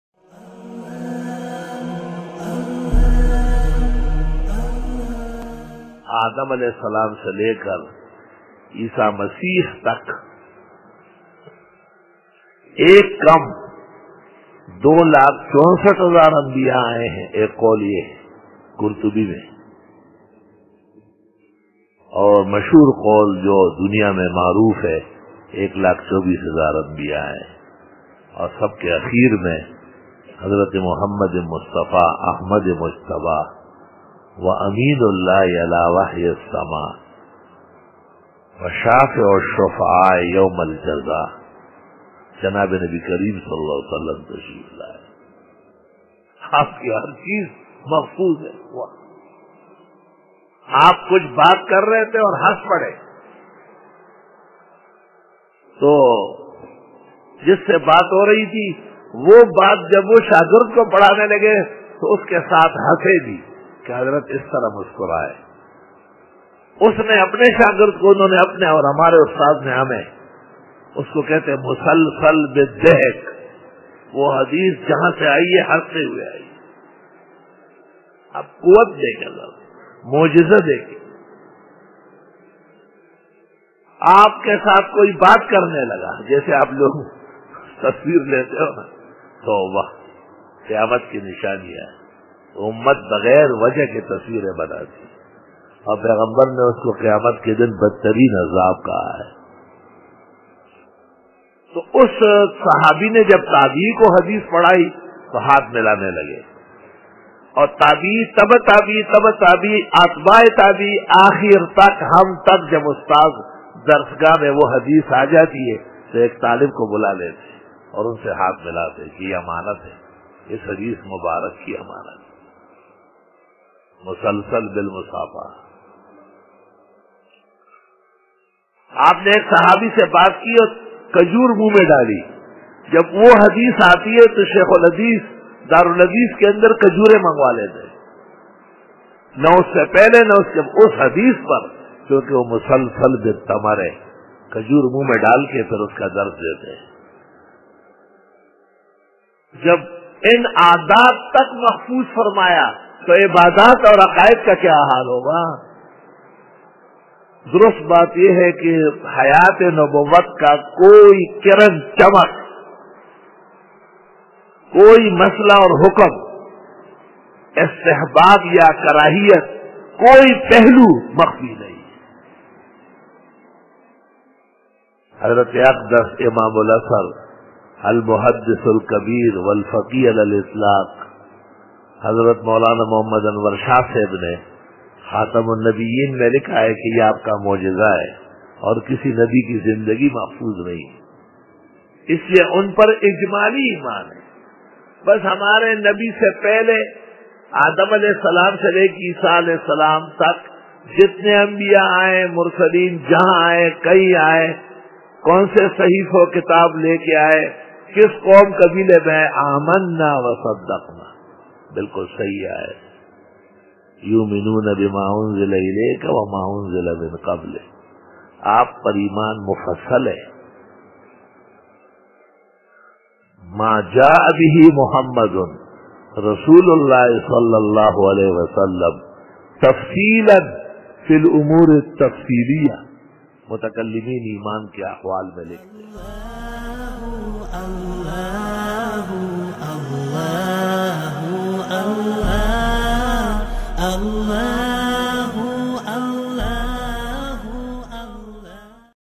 Miracles of Prophet Muhammad SAW | جناب رسول اللہ ﷺ کے معجزات | SC676 | Mufti Zarwali Khan
SC676
Miracles of Prophet Muhammad SAW
جناب رسول اللہ ﷺ کے معجزات
Janab e Rasool SAW k moajjizaat

Bayan by: Molana Mufti Mohammad Zarwali Khan Sahab